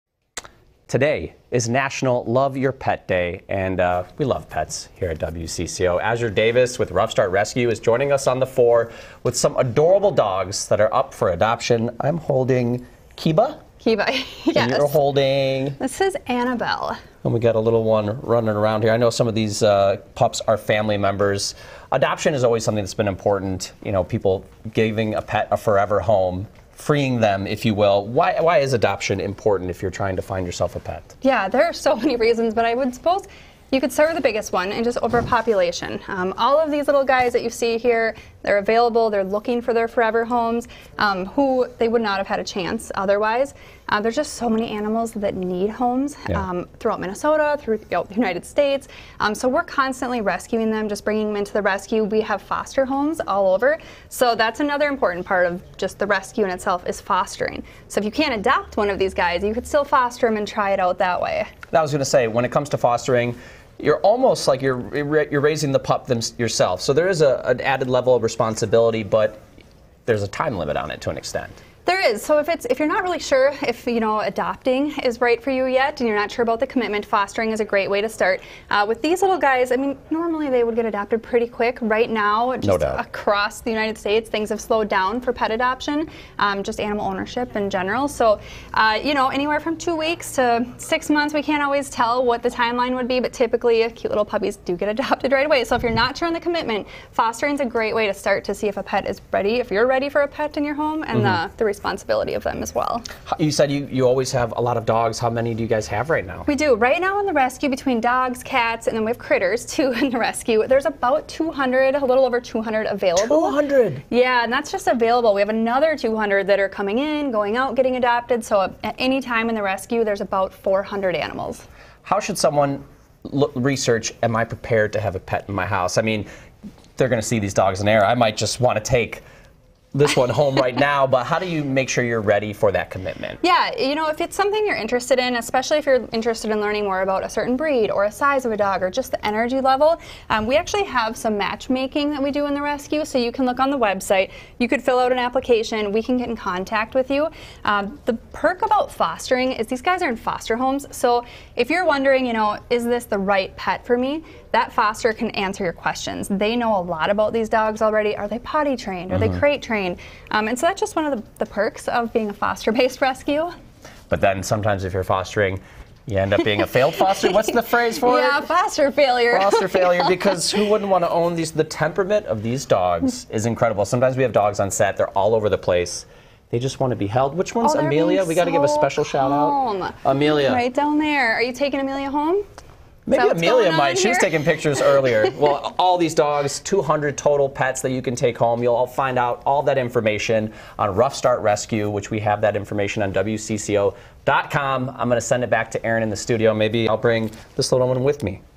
It's National Love Your Pet Day
Azure Davis with Ruff Start Rescue shares some dogs who are currently up for adoption.